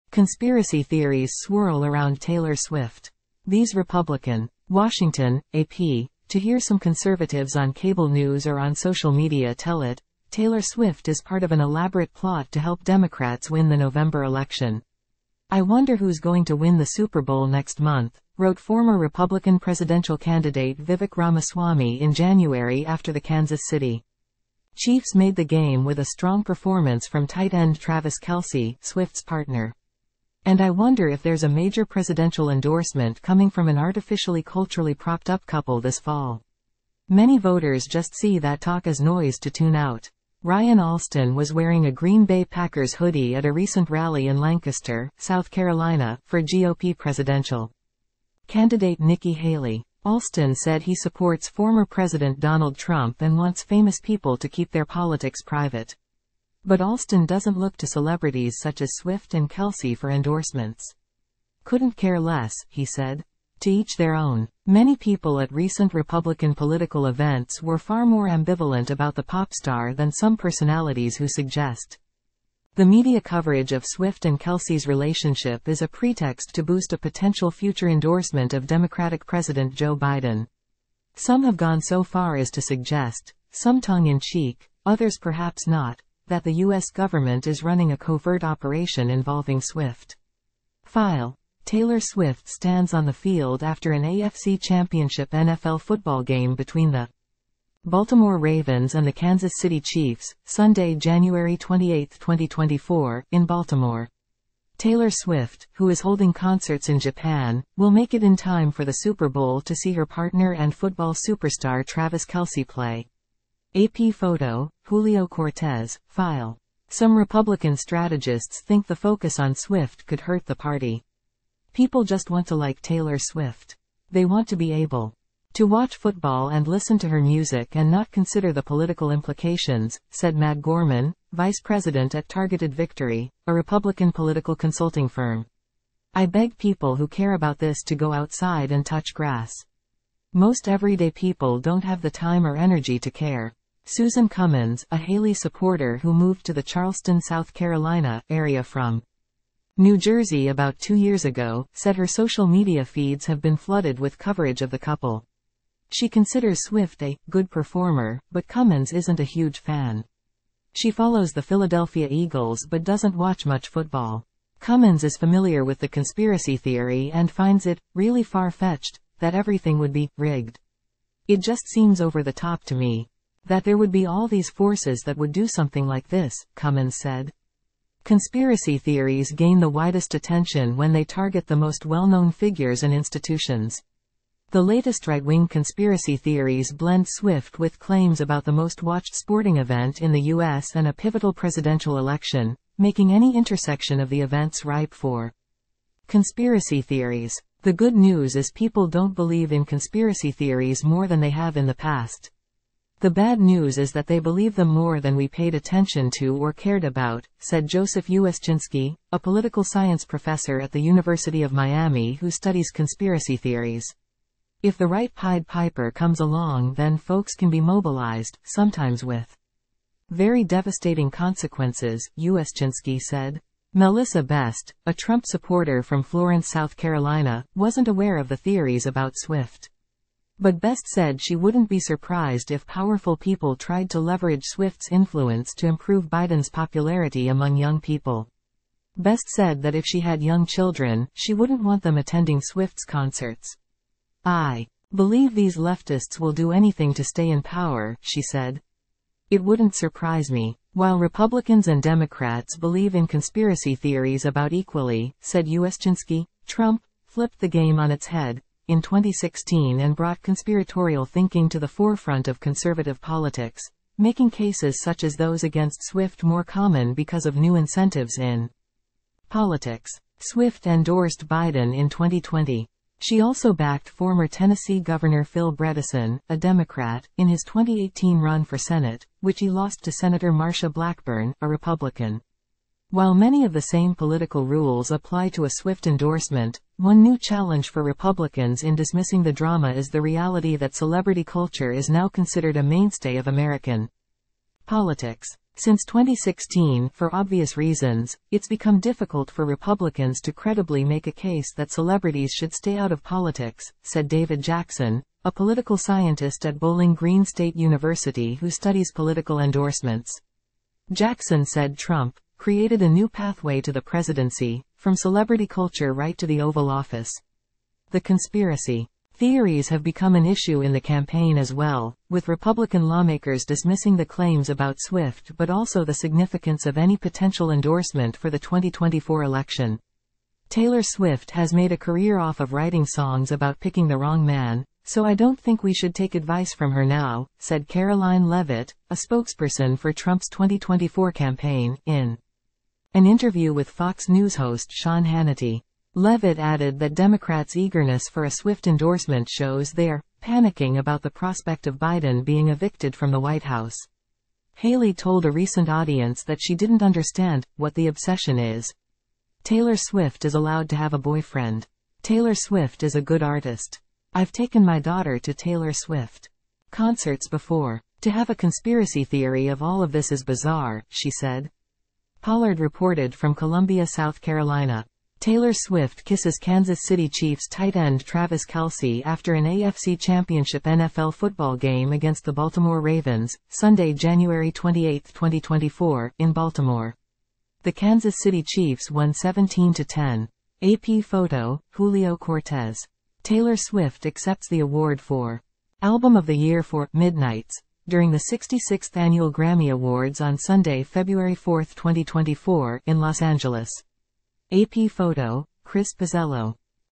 Conspiracy theories swirl around Taylor Swift. These Republican
WASHINGTON (AP) - To hear some conservatives on cable news or on social media tell it, Taylor Swift is part of an elaborate plot to help Democrats win the November election.

"I wonder who´s going to win the Super Bowl next month," wrote former Republican presidential candidate Vivek Ramaswamy in January after the Kansas City Chiefs made the game with a strong performance from tight end Travis Kelce, Swift's partner. "And I wonder if there´s a major presidential endorsement coming from an artificially culturally propped-up couple this fall."

Many voters just see that talk as noise to tune out.

Ryan Allstun was wearing a Green Bay Packers hoodie at a recent rally in Lancaster, South Carolina, for GOP presidential candidate Nikki Haley. Allstun said he supports former President Donald Trump and wants famous people to keep their politics private. But Allstun doesn´t look to celebrities such as Swift and Kelce for endorsements.

"Couldn´t care less," he said. "To each their own."

Many people at recent Republican political events were far more ambivalent about the pop star than some personalities who suggest the media coverage of Swift and Kelce's relationship is a pretext to boost a potential future endorsement of Democratic President Joe Biden. Some have gone so far as to suggest - some tongue in cheek, others perhaps not - that the U.S. government is running a covert operation involving Swift.

FILE - Taylor Swift stands on the field after an AFC Championship NFL football game between the Baltimore Ravens and the Kansas City Chiefs, Sunday, Jan. 28, 2024, in Baltimore. Taylor Swift, who is holding concerts in Japan, will make it in time for the Super Bowl to see her partner and football superstar Travis Kelce play. (AP Photo/Julio Cortez, File)

Some Republican strategists think the focus on Swift could hurt the party.

"People just want to like Taylor Swift. They want to be able to watch football and listen to her music and not consider the political implications," said Matt Gorman, vice president at Targeted Victory, a Republican political consulting firm. "I beg people who care about this to go outside and touch grass. Most everyday people don´t have the time or energy to care."

Susan Cummins, a Haley supporter who moved to the Charleston, South Carolina, area from New Jersey about two years ago, said her social media feeds have been flooded with coverage of the couple. She considers Swift a "good performer," but Cummins isn´t a huge fan. She follows the Philadelphia Eagles but doesn´t watch much football.

Cummins is familiar with the conspiracy theory and finds it "really far-fetched" that everything would be "rigged."

"It just seems over the top to me that there would be all these forces that would do something like this," Cummins said.

Conspiracy theories gain the widest attention when they target the most well-known figures and institutions. The latest right-wing conspiracy theories blend Swift with claims about the most watched sporting event in the U.S. and a pivotal presidential election, making any intersection of the events ripe for conspiracy theories.

"The good news is people don´t believe in conspiracy theories more than they have in the past. The bad news is that they believe them more than we paid attention to or cared about," said Joseph Uscinski, a political science professor at the University of Miami who studies conspiracy theories.

"If the right Pied Piper comes along then folks can be mobilized, sometimes with very devastating consequences," Uscinski said.

Mellissa Best, a Trump supporter from Florence, South Carolina, wasn´t aware of the theories about Swift. But Best said she wouldn´t be surprised if powerful people tried to leverage Swift´s influence to improve Biden´s popularity among young people. Best said that if she had young children, she wouldn´t want them attending Swift´s concerts.

"I believe these leftists will do anything to stay in power," she said. "It wouldn´t surprise me."

While Republicans and Democrats believe in conspiracy theories about equally, said Uscinski, Trump "flipped the game on its head" in 2016 and brought conspiratorial thinking to the forefront of conservative politics, making cases such as those against Swift more common because of new incentives in politics.

Swift endorsed Biden in 2020. She also backed former Tennessee Gov. Phil Bredesen, a Democrat, in his 2018 run for Senate, which he lost to Sen. Marsha Blackburn, a Republican.

While many of the same political rules apply to a Swift endorsement, one new challenge for Republicans in dismissing the drama is the reality that celebrity culture is now considered a mainstay of American politics.

"Since 2016, for obvious reasons, it´s become difficult for Republicans to credibly make a case that celebrities should stay out of politics," said David Jackson, a political scientist at Bowling Green State University who studies